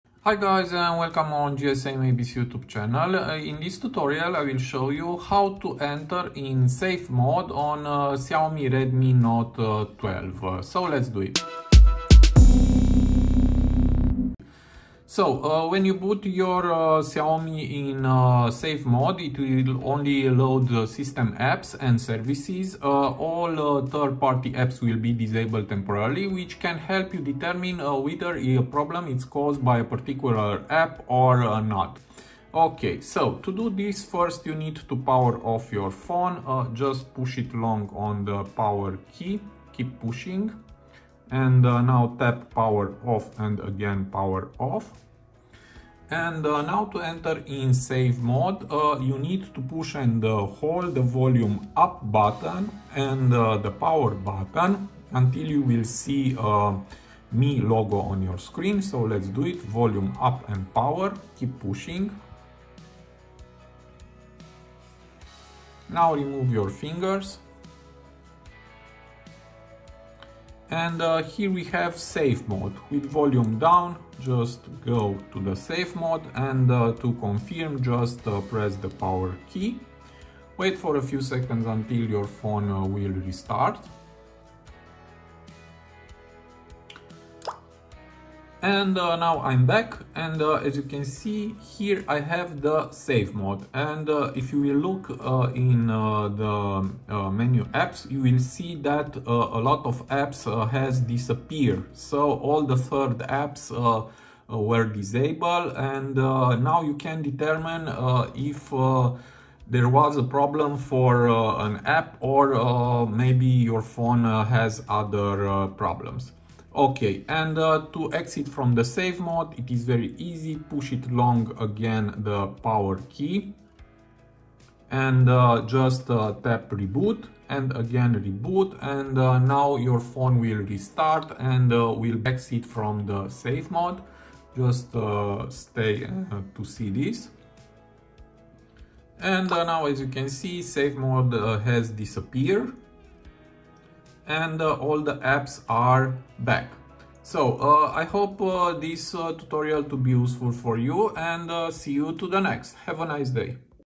How to Enable SAFE MODE on XIAOMI Redmi Note 12
In this video I'll show you how to enable safe mode on Xioami Redmi Note 12 & 12 Pro.
Safe mode can be useful on a Xiaomi Redmi Note 12 if you are experiencing issues with the phone that you suspect may be caused by a third-party app. By booting the device into safe mode, you can temporarily disable all third-party apps and only run the basic system apps and services. This can help you determine whether the problem is caused by an app or by the system itself.

If your phone is behaving unusually, such as crashing, freezing, or running slowly, it may be due to a problematic app. Booting your Redmi Note 12 into safe mode will help you identify which app is causing the issue, so you can uninstall it or troubleshoot it accordingly.

Safe mode can also be helpful if you suspect that your phone has been infected by malware or a virus. By booting into safe mode, you can prevent any malicious apps from running and take steps to remove them.